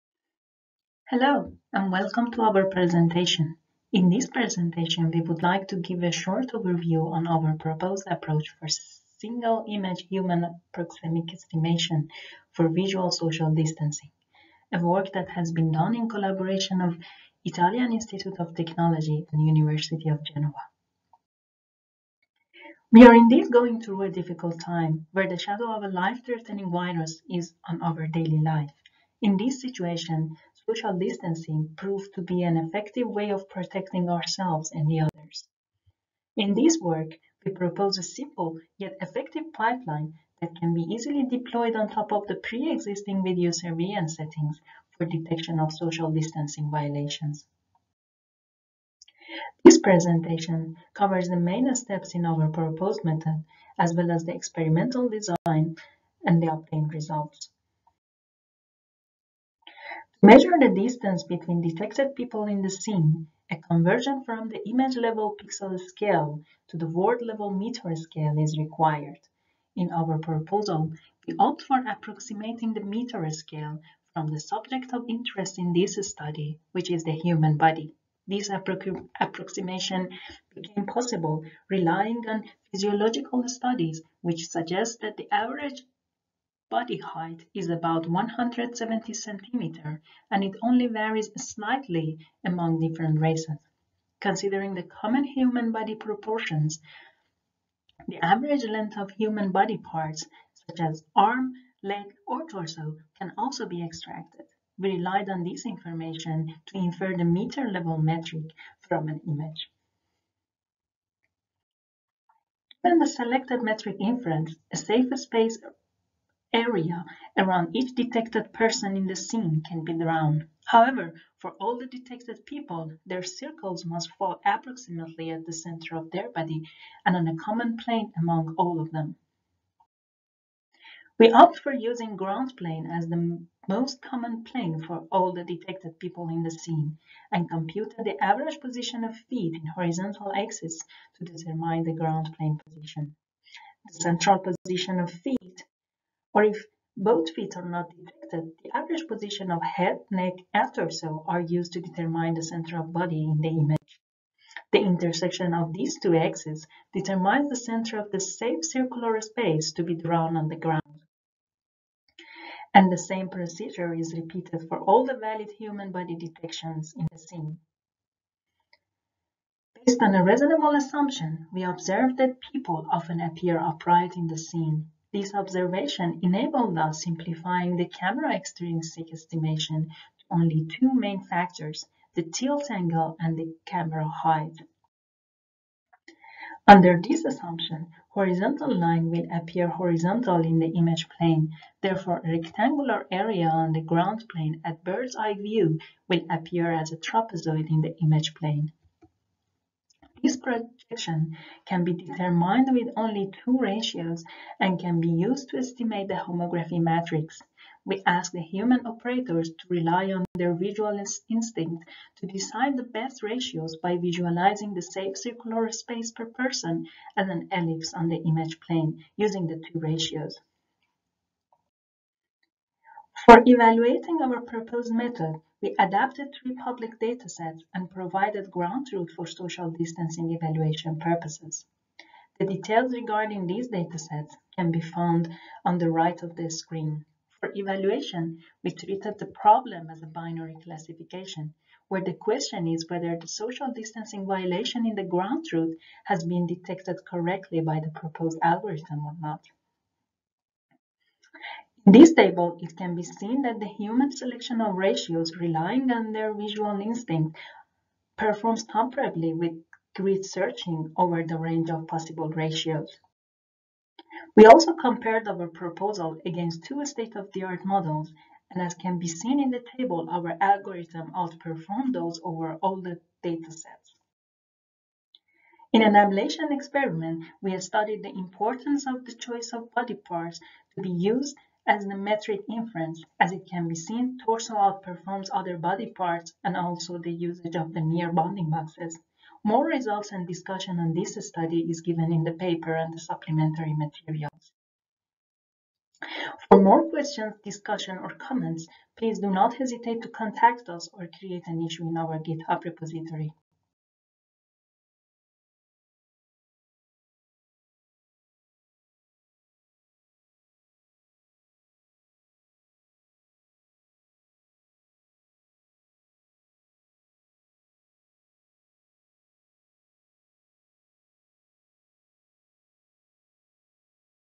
[WACV 2021] Single Image Human Proxemics Estimation for Visual Social Distancing
We are going through a difficult time, where the shadow of a life-threatening virus is on our daily life. In this situation, social distancing proved to be an effective way of protecting ourselves and others. In this work we propose a simple, yet effective pipeline that can be easily deployed on top of the pre-existing video surveillance settings for detection of social distancing violations. This work has been done in collaboration with my colleagues in Istituto Italiano di Tecnologia, among whom is Maya Aghaei, the leading author.